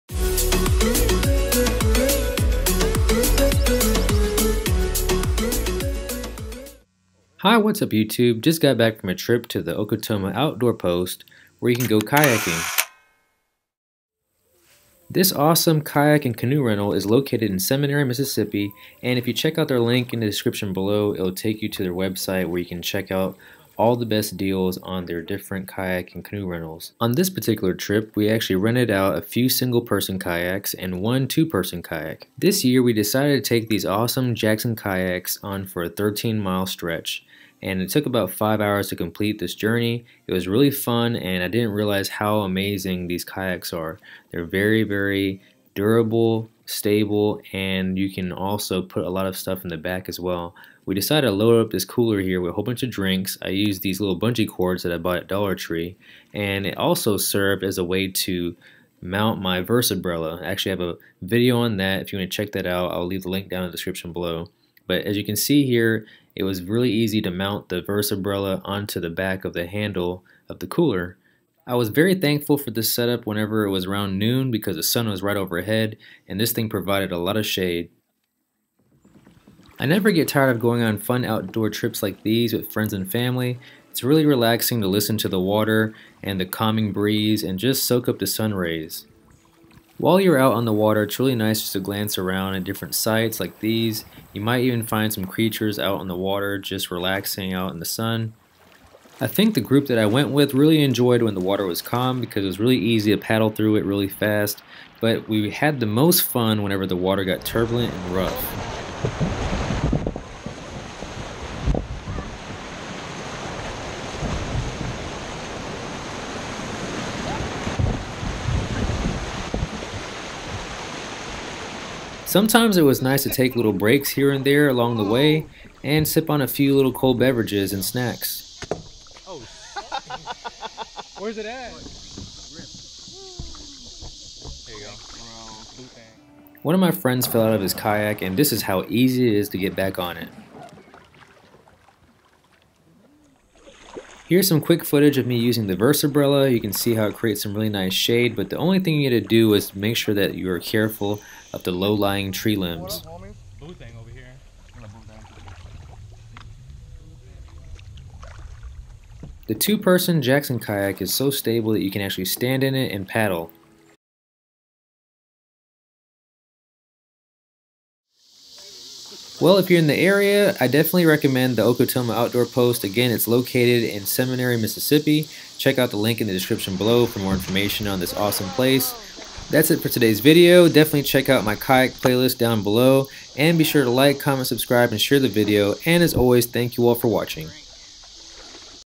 🛶2 Person Kayak Fun Trip at Okatoma Outdoor Post | Jackson Kayaks are Stable & Easy to Maneuver
☂️Best Kayak Umbrella ➜

I made another trip to Okatoma Outdoor Post and rented a 2 person kayak. It was very fun, stable, and had room to carry a small cooler in the back. We decided to go on the longest kayak trail. Usually we do the 7mile trip that takes about 3 hours. This year we went on the 13 mile trip that took about 5 hours but was so fun! I hope you can learn some tips from my kayak set up and see how easy it is to get back in a kayak when you fall out. The single person Kia can be held by one person but the two person kayak definitely needs to people to pick it up although one person can drag it but that’s not good for the kayak.